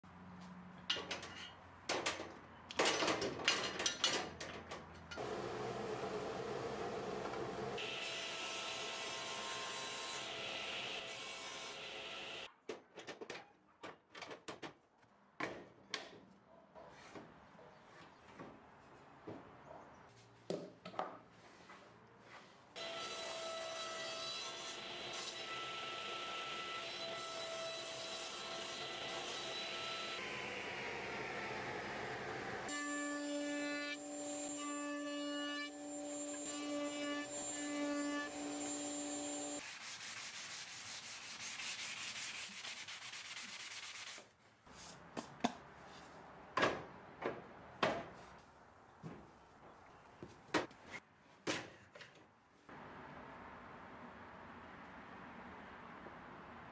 Custom Shelves for Wall Control Pegboard
Using this simple method for custom shelves with the Wall Control pegboard system allows for tons of customizability.

KNEW CONCEPTS FRET SAW
Edgebanding Trimmer
Shinwa Square
Dewalt Router
Precision T-rule
Machine Square
FESTOOL Sortainer
BESSEY Clamps